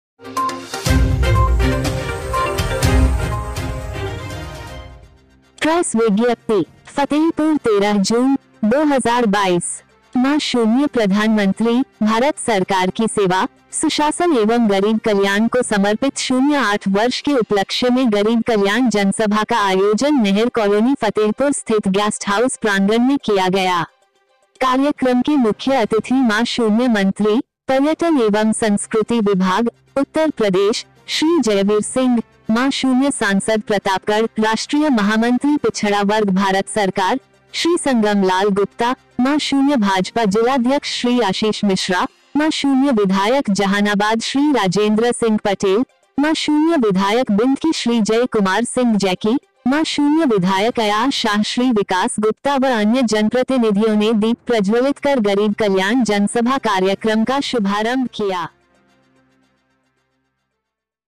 फतेहपुर 13 जून,2022मा0 प्रधानमंत्री, भारत सरकार की सेवा, सुशासन एवं गरीब कल्याण को समर्पित 08
प्रेस विज्ञप्ति
फतेहपुर 13 जून,2022
मा0 प्रधानमंत्री, भारत सरकार की सेवा, सुशासन एवं गरीब कल्याण को समर्पित 08 वर्ष के उपलक्ष्य में *गरीब कल्याण जनसभा* का आयोजन नहर कालोनी फतेहपुर स्थित गेस्ट हाउस प्रांगण में किया गया । कार्यक्रम के मुख्य अतिथि मा0 मंत्री, पर्यटन एवं संस्कृति विभाग, उत्तर प्रदेश, श्री जयवीर सिंह, मा0 सांसद प्रतापगढ़/राष्ट्रीय महामंत्री पिछड़ा वर्ग भारत सरकार, श्री संगम लाल गुप्ता, मा0 भाजपा जिलाध्यक्ष श्री आशीष मिश्रा, मा0 विधायक जहानाबाद श्री राजेन्द्र सिंह पटेल, मा0 विधायक बिन्दकी श्री जय कुमार सिंह जैकी, मा0 विधायक अयाह शाह श्री विकास गुप्ता व अन्य जनप्रतिनिधियों ने दीप प्रज्वलित कर *गरीब कल्याण जनसभा* कार्यक्रम का शुभारंभ किया ।
   मुख्य अतिथि मा0 मंत्री, पर्यटन एवं संस्कृति विभाग, उत्तर प्रदेश, श्री जयवीर सिंह ने कार्यक्रम को संबोधित करते हुए कहा कि केन्द्र सरकार व राज्य सरकार द्वारा गरीब के कल्याणार्थ संचालित योजनाओं में महिलाओं को प्रथम वरीयता के आधार पर लाभान्वित किया जा रहा है । उन्होंने कहा कि मा0 प्रधानमंत्री भारत सरकार श्री नरेन्द्र मोदी जी व मा0 मुख्यमंत्री उ0प्र0 सरकार श्री योगी आदित्यनाथ जी महिलाओं के सशक्त व सम्मान के लिए दृढ़ संकल्पित है। भारत सरकार द्वारा महिलाओं को धुएरहित खाना पकाने के लिए प्रधानमंत्री उज्ज्वला योजनांतर्गत निःशुल्क गैस कनेक्शन देकर धुँए से छुटकारा दिलाया है । उन्होंने कहा कि हमारी डबल इंजन की सरकार पंडित दीनदयाल उपाध्याय जी के सपने को साकार करने का काम बिना भेदभाव के कर रही है कि समाज के अंतिम पायदान में खड़े पात्र व्यक्ति को योजना का लाभ मिल रहा है । प्रदेश व देश विकास की ओर अग्रसर है। भारत के संविधान के अनुसार सभी को बिना भेदभाव के सम्मान व न्याय दिया जा रहा है। गरीबो के कल्याण के लिए हमारी डबल इंजन कि सरकार दिन रात कार्य करके सामाजिक व आर्थिक रूप से नागरिको को समृद्ध बनाने का कार्य किया जा रहा है । कोविड-19 महामारी के दौरान डबल इंजन की सरकार ने गरीब कल्याण योजनांतर्गत निःशुल्क खाद्यान्न उपलब्ध कराया, किसी को भूखे पेट नही सोने दिया गया, इसमें हमारे कार्यकर्ताओं ने अहम भूमिका निभाई है । हमारी सरकार का सपना है कि कोई भी व्यक्ति अब झोपड़ी में नही रहेगा, हर पात्र व्यक्ति के पास अपनी खुद की पक्की छत होगी । जनपद फतेहपुर की धरती क्रांतिकारियों की है इसका हम सम्मान करते है, गांव की गांव तक की संस्कृति और परंपराओं व पर्यटन को बढ़ावा देने हेतु पर्यटन एवं संस्कृति विभाग अपेक्षित सहयोग देगा ।
  मा0 सांसद प्रतापगढ़/राष्ट्रीय महामंत्री पिछड़ा वर्ग भारत सरकार, श्री संगम लाल गुप्ता ने अपने संबोधन में कहा कि डबल इंजन की सरकार ने प्रदेश को गरीबी मुक्त, अपराधमुक्त, भ्रष्टाचारमुक्त बनाने का कार्य कर रही है । गरीब शोषित,  वंचित, समाज के अतिंम पायदान के पात्र व्यक्ति को समृद्ध बनाने के लिए बिना भेदभाव के केन्द्र सरकार व राज्य सरकार की योजनाओ का लाभ दिया जा रहा है। उन्होंने कहा कि जब गांव विकसित होगा तभी प्रदेश व देश विकसित होगा इसलिए डबल इंजन की सरकार गांवों को विकसित करने का प्रयास कर रही है। हमारी सरकार सबका साथ, सबका विकास, सबका विश्वास व सबके प्रयास से बिना भेद भाव के योजनाओ से संतृप्त किया जा रहा है । समाज के हर व्यक्ति को संविधान के अनुसार न्याय व सम्मान दिया जा रहा है । प्रदेश में कानूनराज स्थापित है समाज भयमुक्त है ।
    *इस मौके पर मा0 मंत्री जी व जनप्रतिनिधियों द्वारा श्रीमती मंजू देवी पत्नी अखिलेश लोधी ब्लाक तेलियानी, श्रीमती ममता देवी पत्नी अमरपाल ब्लॉक तेलियानी, श्रीमती केसिया पत्नी गोलहा ब्लॉक मलवां, श्रीमती प्रेमता देवी पत्नी सियाराम ब्लाक मलवां, श्रीमती संगीता पत्नी हरीशचंद्र ब्लाक हसवा को प्रधानमंत्री आवास योजनांतर्गत आवास की चाबी एवं स्वयं सहायता समूह चांदनी महिला प्रेरणा संकुल श्रीमती सोनम सिंह, शिवकली, तिरंगा महिला संकुल समिति की शालिनी, सीता महिला ग्राम संगठन श्रीमती रीता देवी, दिशा संकुल समिति श्रीमती सरोज कला, विमला तिवारी को ₹ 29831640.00【दो करोड़, अट्ठानवे लाख, इकत्तीस(31) हजार, छः सौ चालीस रुपये】का प्रतीकात्मक चेक देकर महिलाओं को सशक्त बनाने का कार्य किया । जिससे आत्मनिर्भर होकर अपना और अपने परिवार का विकास कर सकती है ।
    कार्यक्रम का सफल संचालन मा0 भाजपा जिलाउपाध्यक्ष, श्रीमती अपर्णा सिंह गौतम ने किया ।
        इस अवसर पर मा0 भाजपा पूर्व जिलाध्यक्ष श्री प्रमोद द्विवेदी, पुष्पराज पटेल जिला महामंत्री, नीरज सिंह जिला महामंत्री, अर्चना त्रिपाठी, कुलदीप भदौरिया, मनोज मिश्रा, रेखा मिश्रा, सुशीला गौतम जिला महामंत्री सहित अनेक भाजपा के पदाधिकारी व उपजिलाधिकारी सदर नन्द प्रकाश मौर्य, क्षेत्राधिकारी पुलिस नगर डीसी मिश्र सहित भारी संख्या में लाभार्थी उपस्थित रहे ।